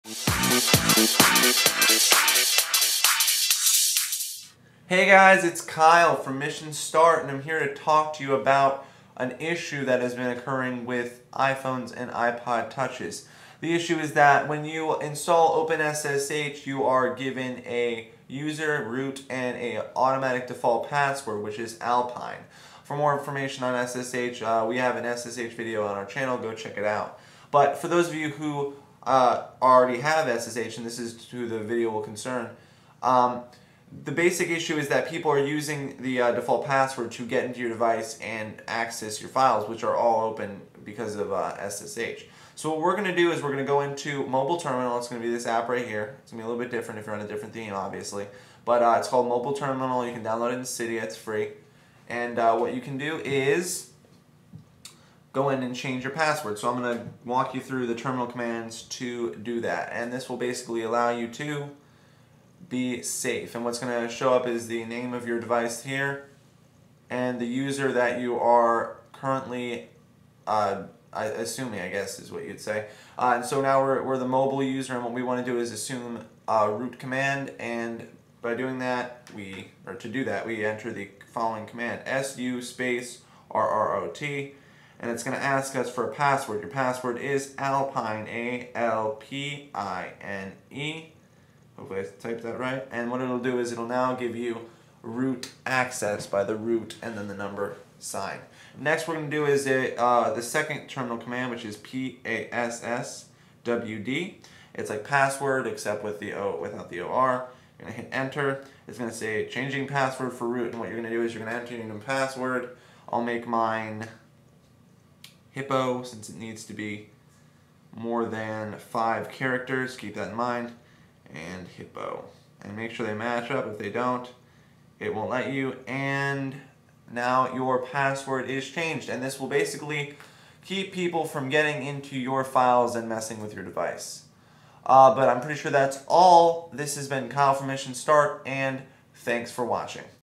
MALWARE 4 iPhone & iPod Touch: How To Prevent It
a simple change of your ssh password will help safe guard you against vicious attacks.